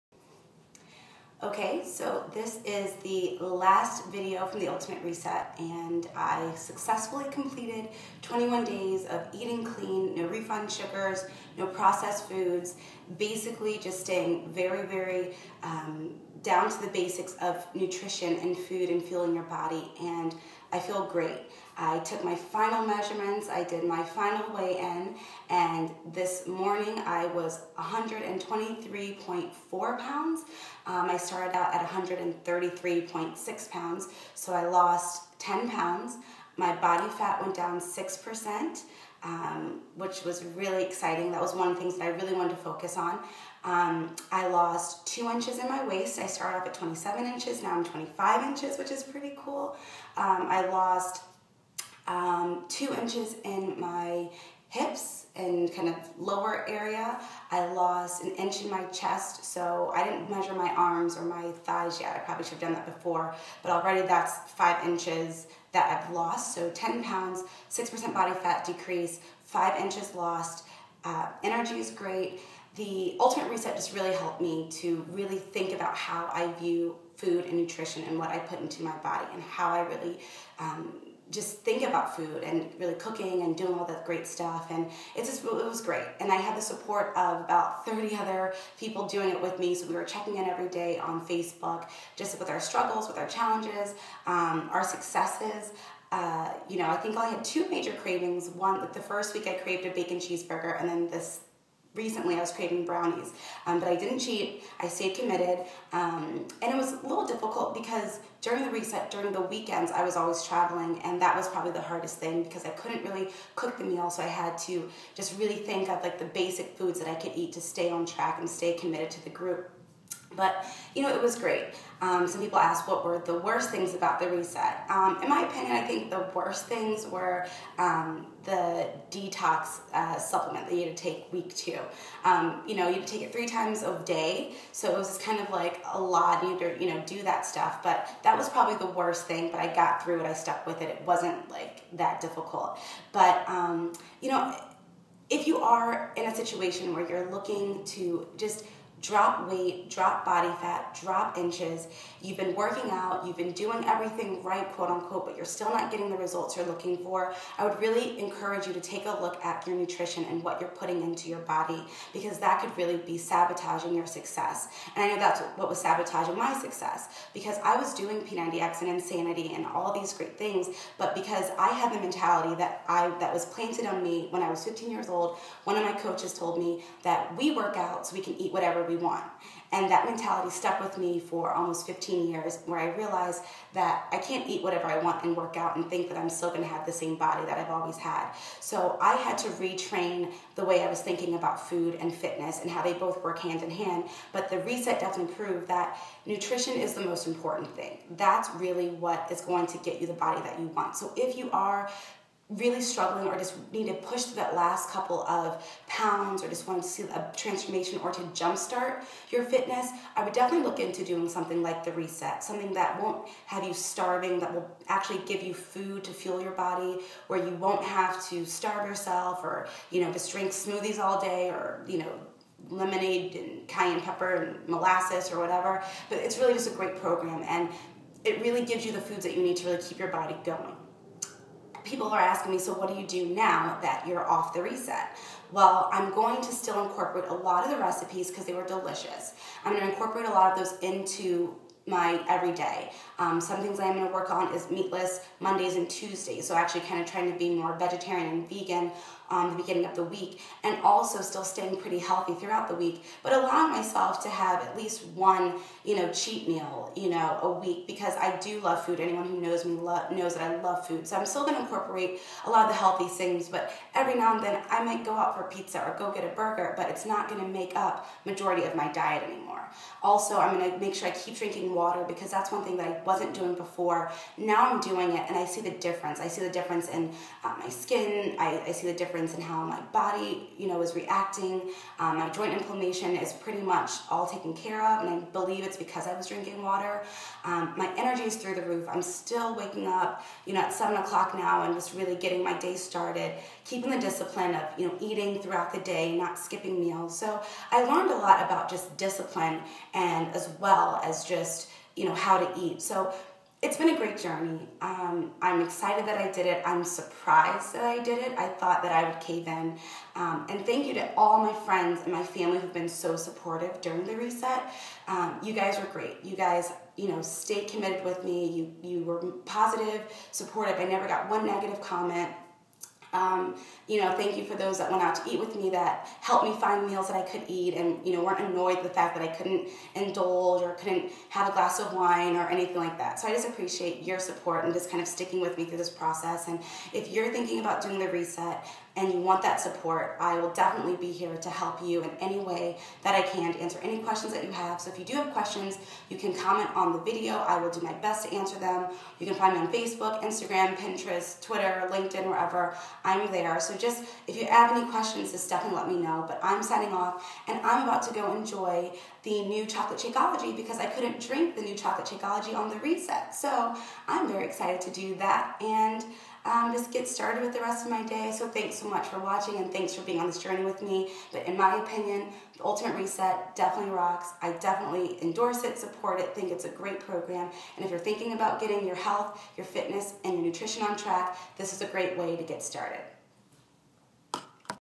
I Survived The Ultimate Reset! Week 3 Recap
Well, I successfully completed the Ultimate Reset.  I can't believe I did it and didn't cheat, but I must say that I feel pretty accomplished! This is a great way to start the new year off! I share my highlights and my least favorite things about the Reset and my plans for continuing to stay on track with my diet and nutrition.